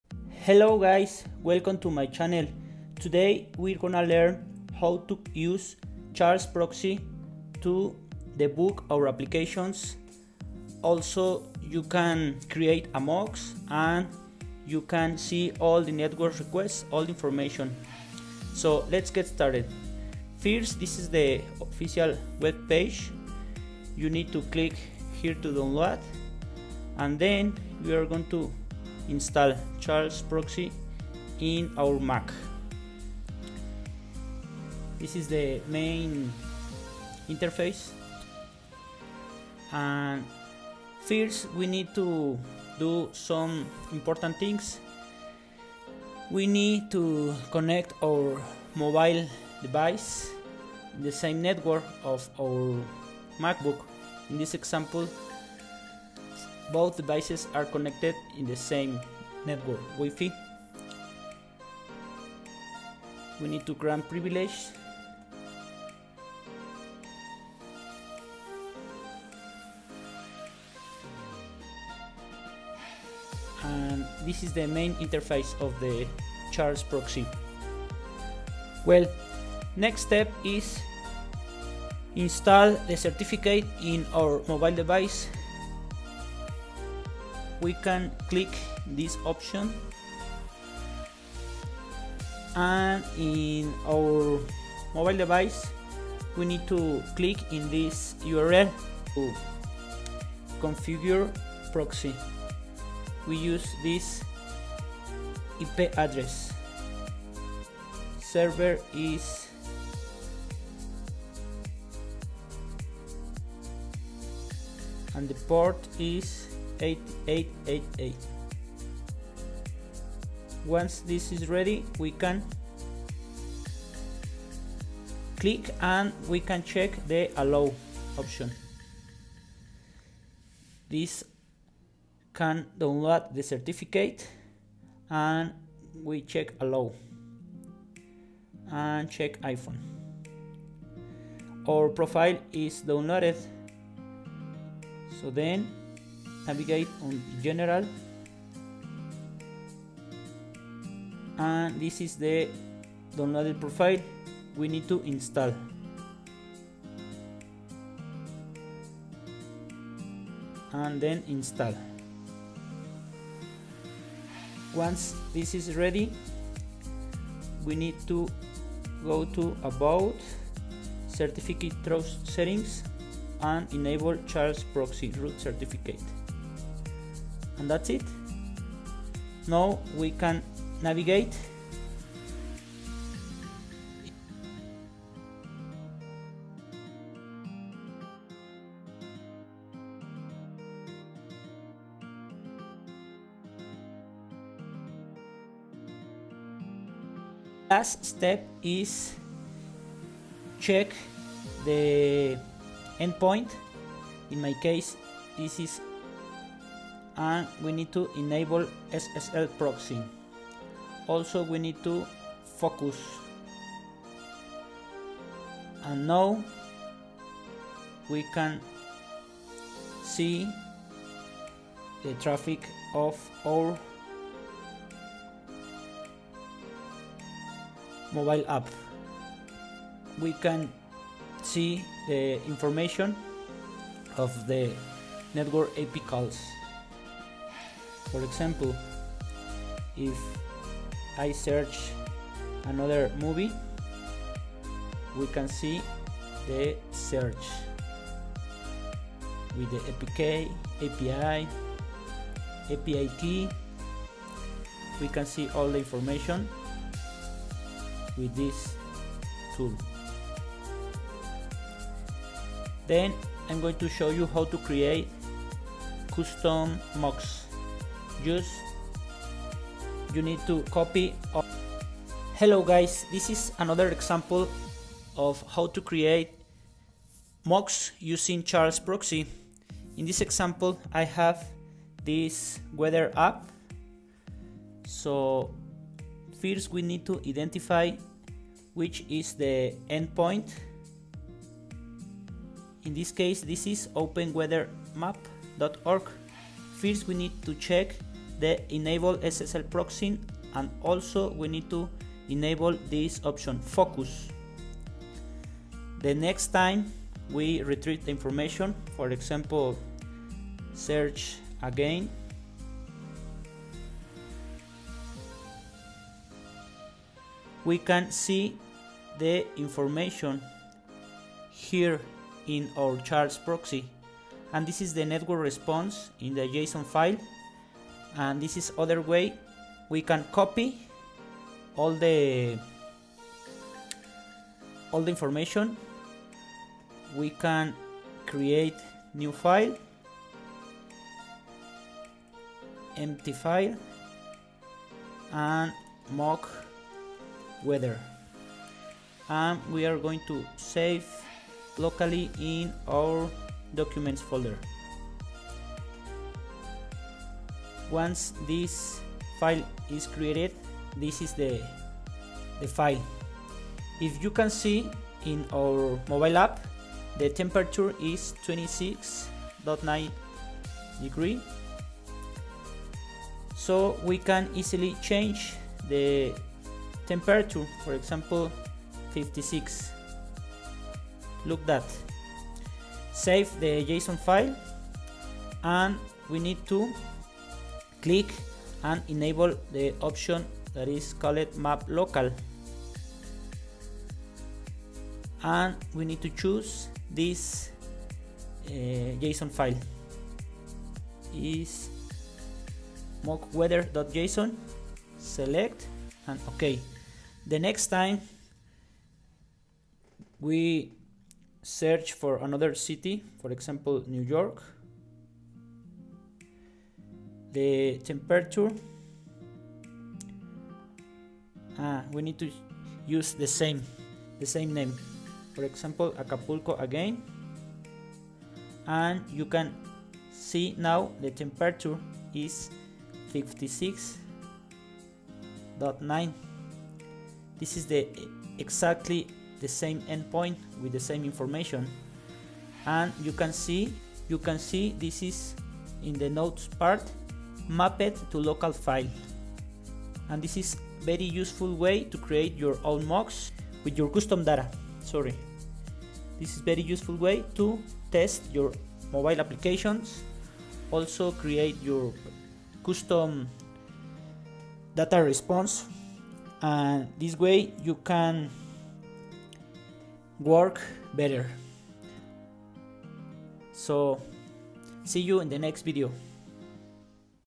How to use and install Charles Proxy on iPhone to analyze Rest API request traffic and create mocks
How to use and install Charles Proxy on iPhone14 to analyze Rest API request traffic and create